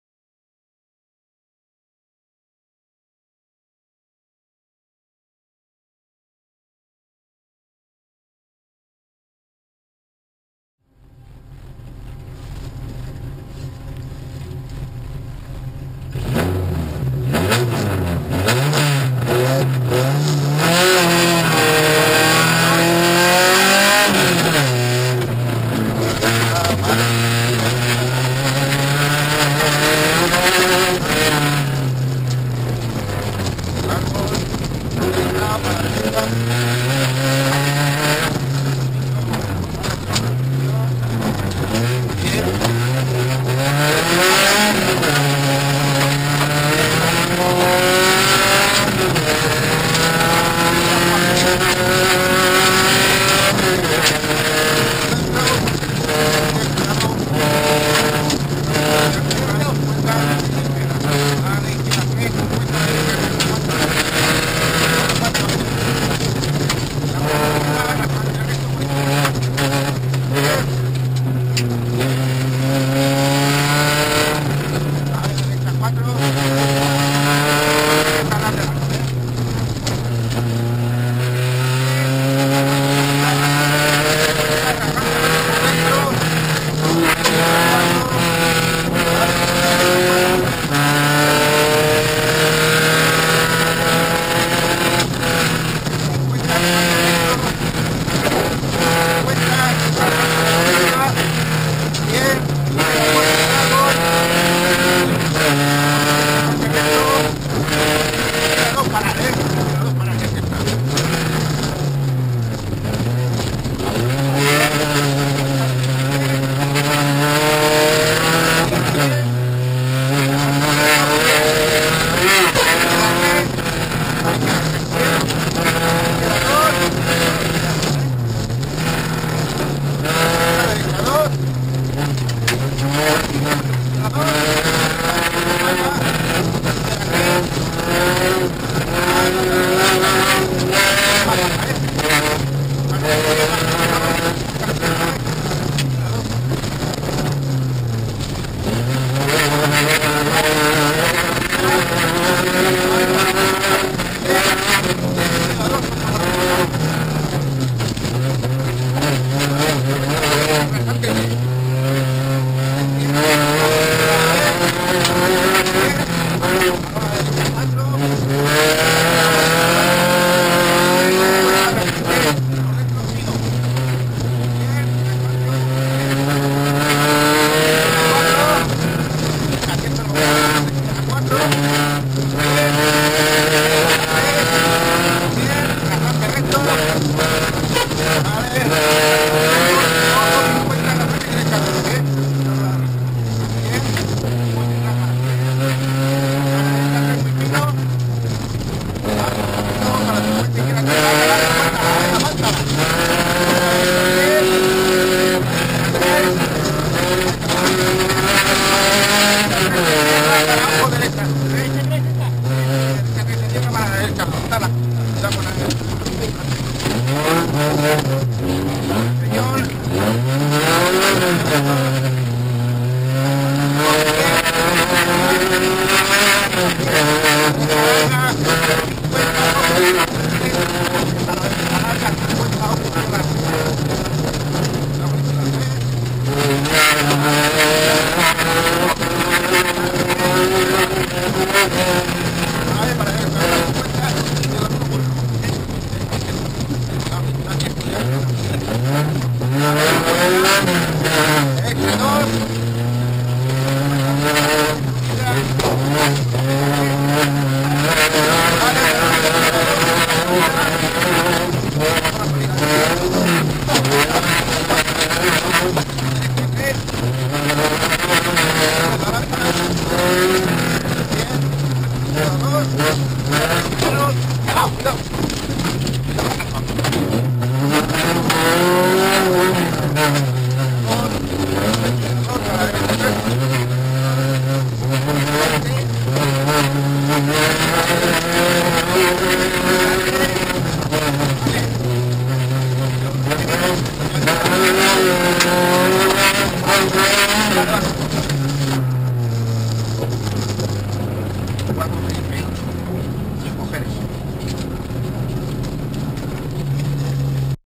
Camara on Board Arellano-Alonso
Tramo Cronometrado en Sevilla la Nueva 2012
3 Pasada